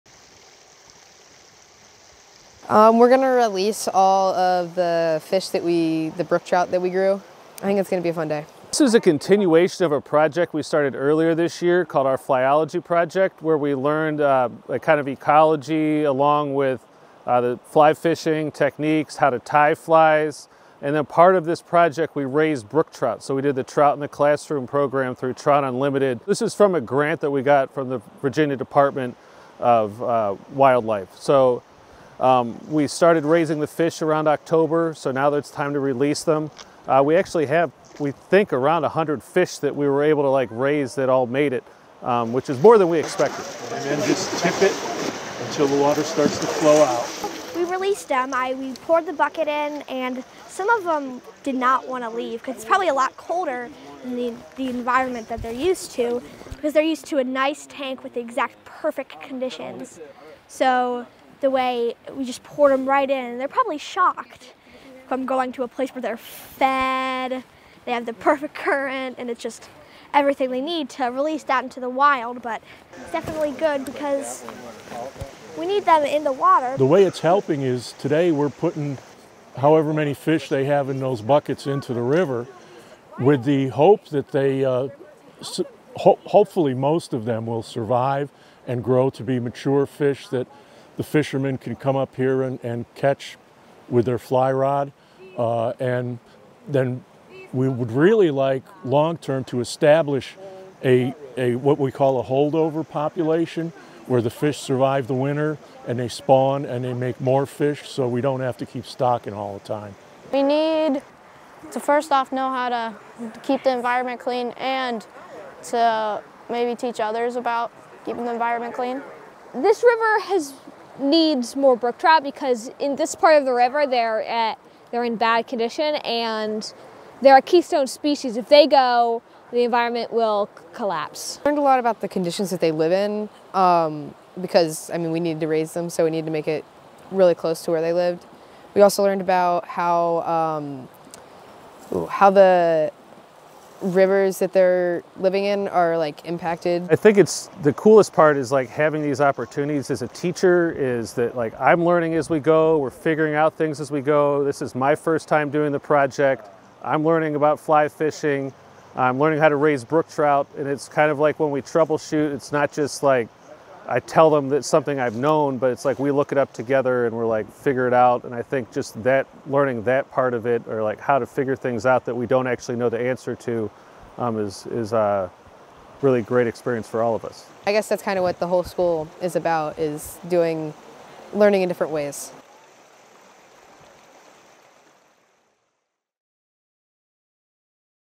Community Lab school Students Release Brook Trout In Moormans River
Students at Community Lab School have been raising brook trout in their science class since October and, during March, they released about 100 brook trout into the Moormans River in Crozet. The students raised the fish as part of their Flyology Project during which they learned about the local ecology, how to tie a fly, and — maybe most importantly — how to fly fish.